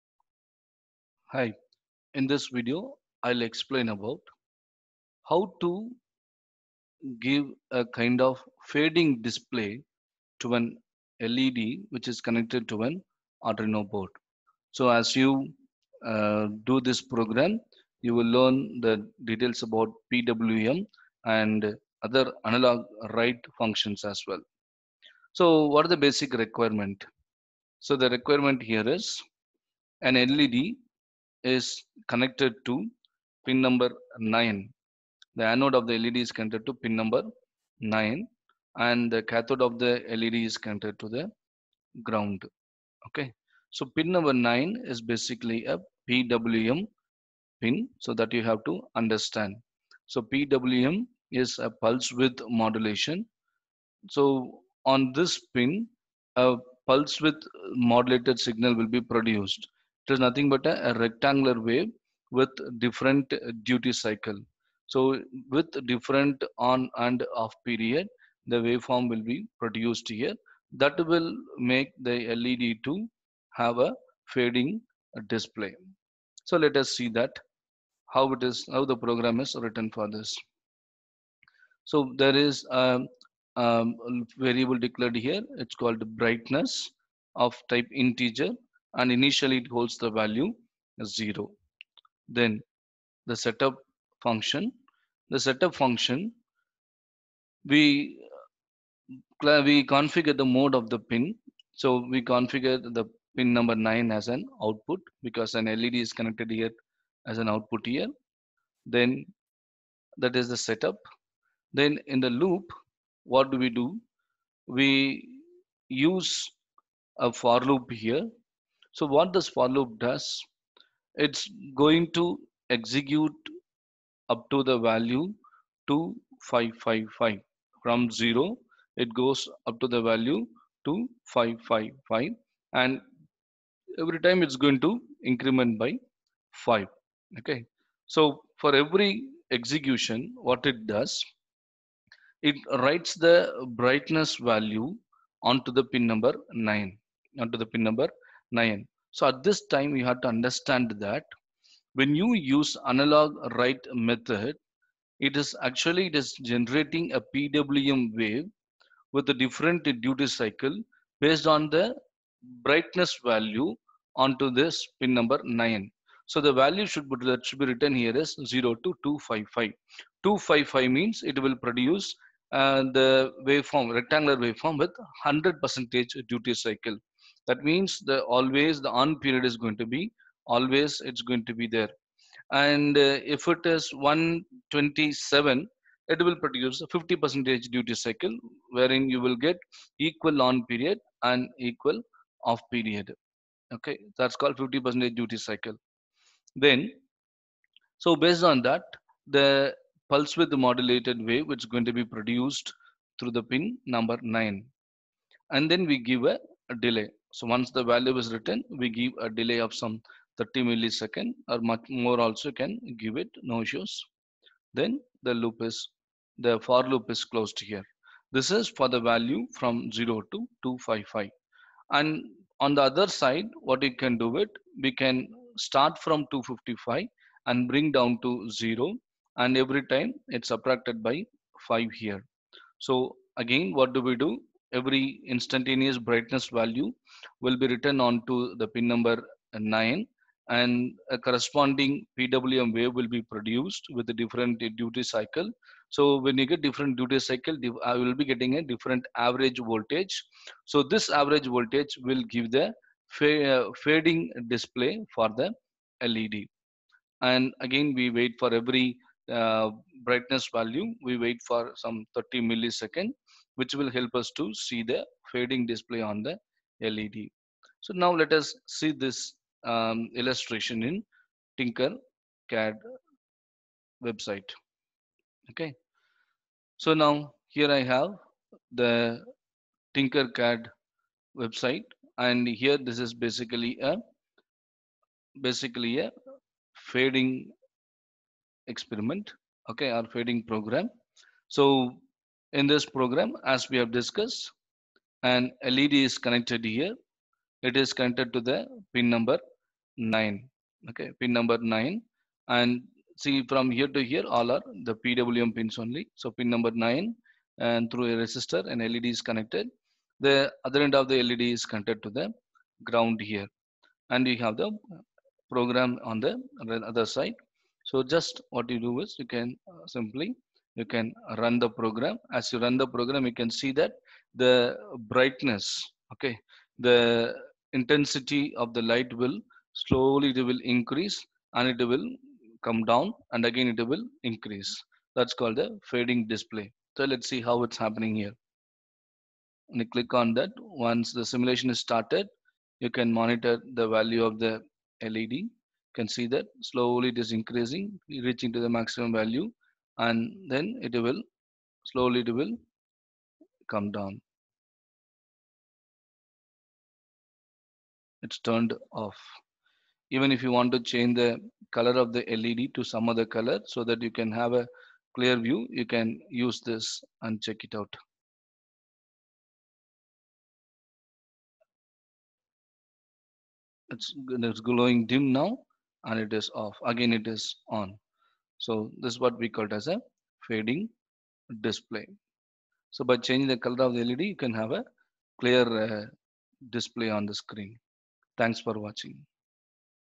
Fade- Fading LED display using Arduino sketch and tinkercad
This video explains about developing arduino sketch for fading LED display  and simulation using Tinkercad